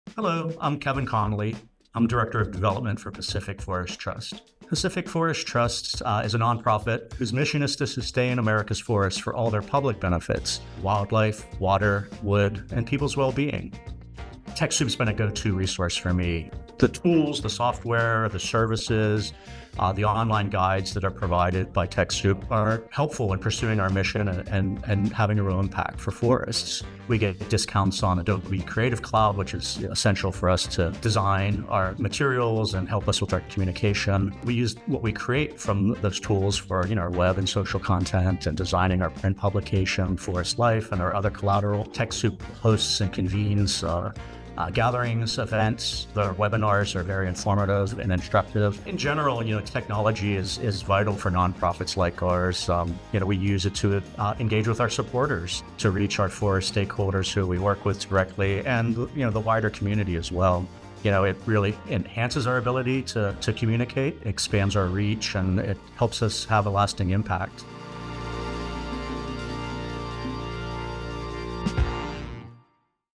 TechSoup Impact Stories: Pacific Forest Trust
With the help of TechSoup, Pacific Forest Trust has been able to effectively conserve forested lands for all the benefits they provide.